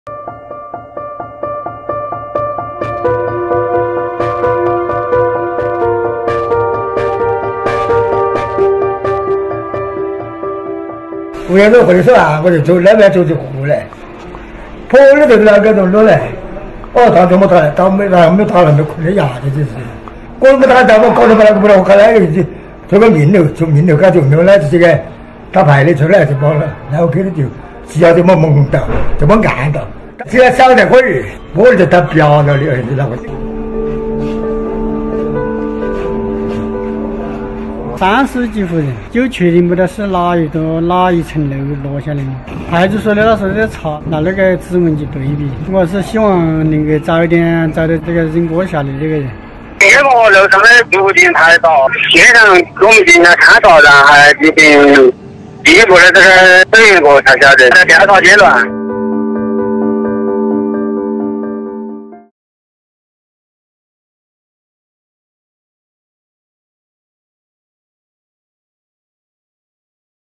Moment elderly man is hit in the head by a flying wok in China
An elderly man was nearly knocked out by a falling wok while he was walking beneath a residential building in southwest China. The 81-year-old pensioner, known as Wang Juqing, suffered bleeding in the head and had to send to hospital by onlookers immediately. Police conducted fingerprint match tests across all residents in an 18-storey building in a bid to track down the culprit.